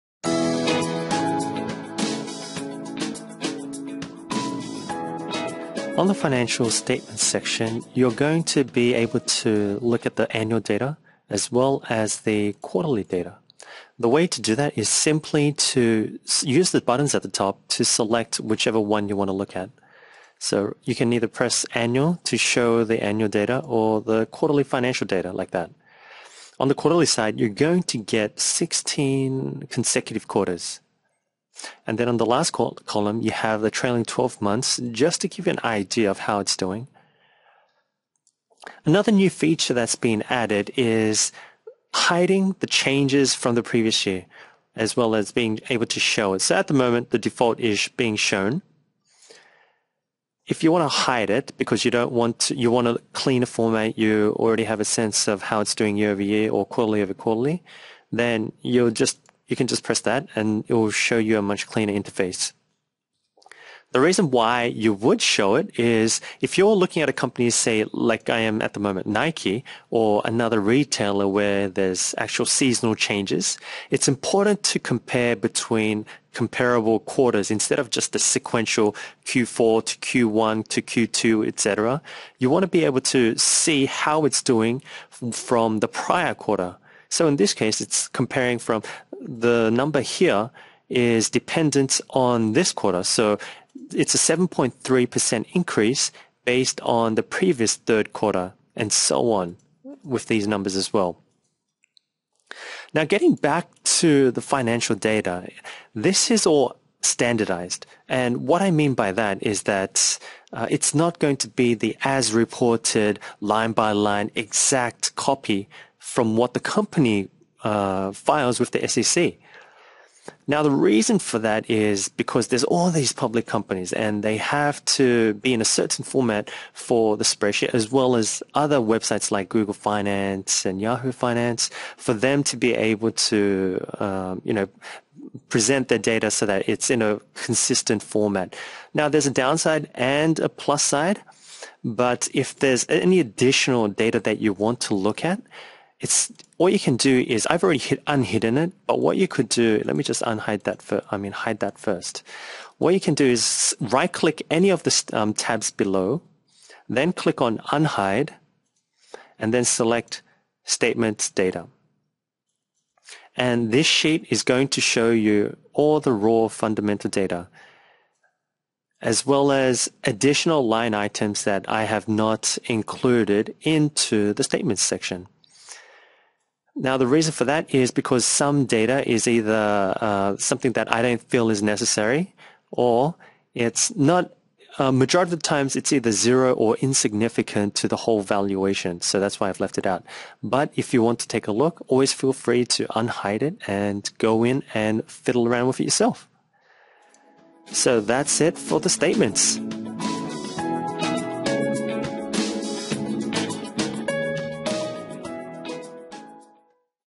Tour of the Automatic Data Pulling Financial Statements
The financial statements automatically retrieves 10 years as well as 16 quarters of financial data.

It's a standardized version so that it is consistent across all the companies you look at.

With the 16 quarterly statements, you can now quickly the changes between comparable quarterly periods. This will help with analyzing seasonal changes.